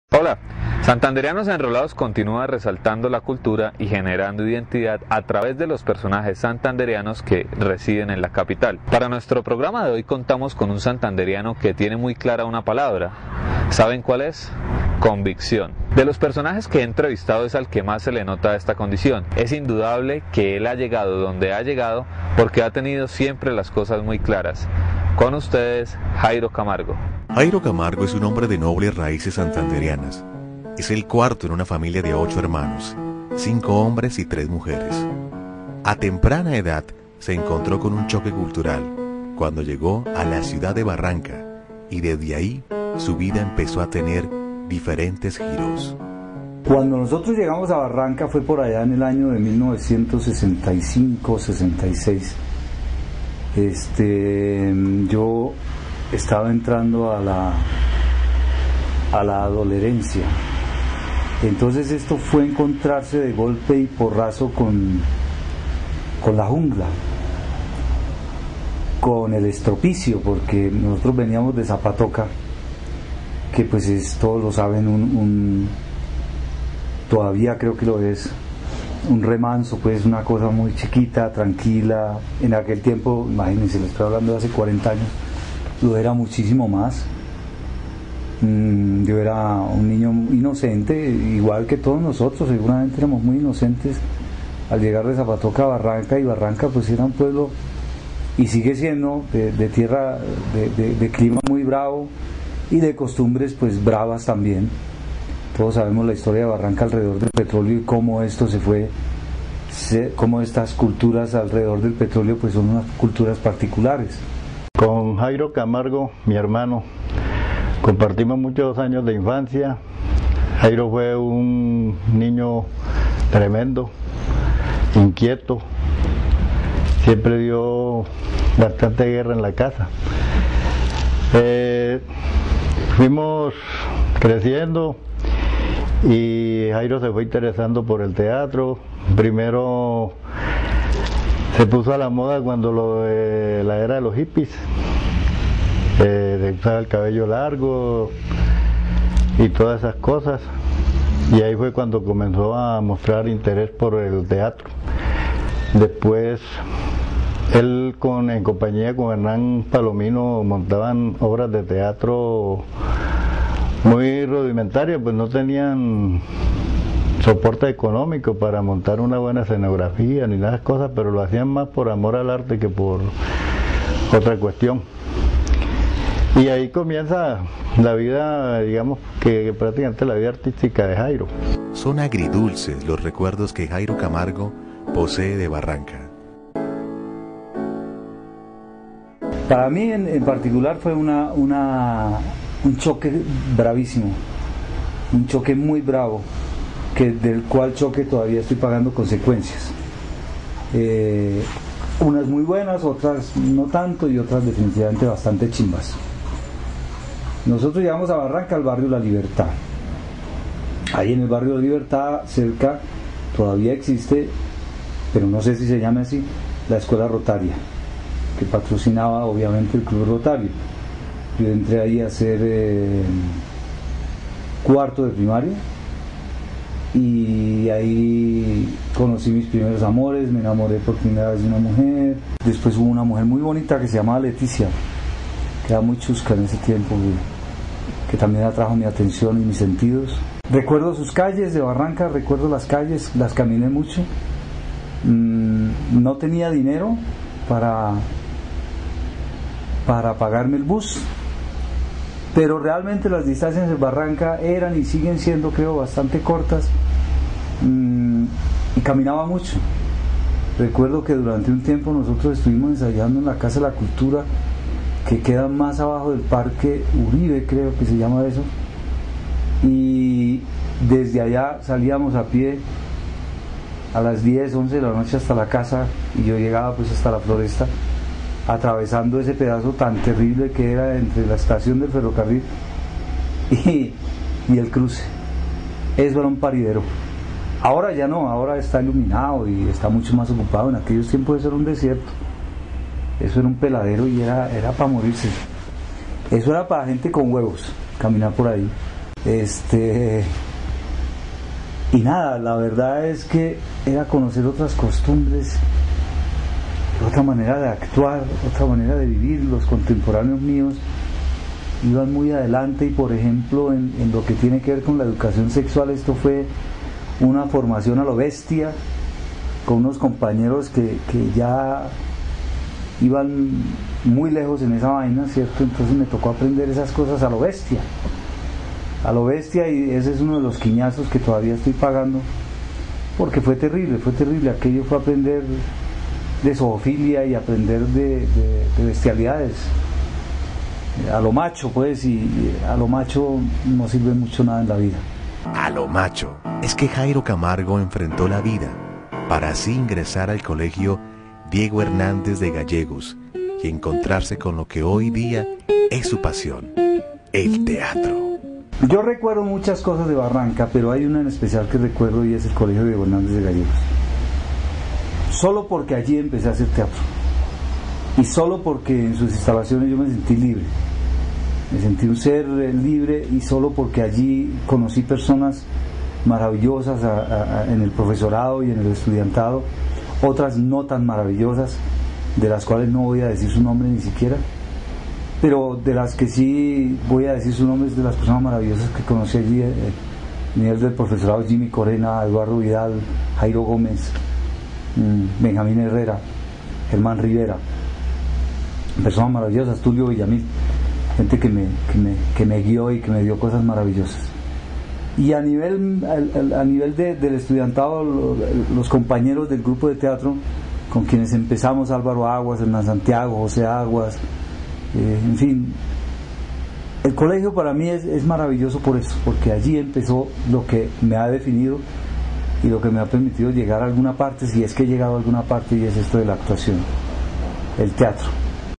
JAIRO CAMARGO SU VIDA
A MI ME PARECIO UN VACAN!
CONFESÓ GRANDES SECRETOS DE SU JUVENTUD
Y TIENE UN HUMOR NEGRO MORDAZ!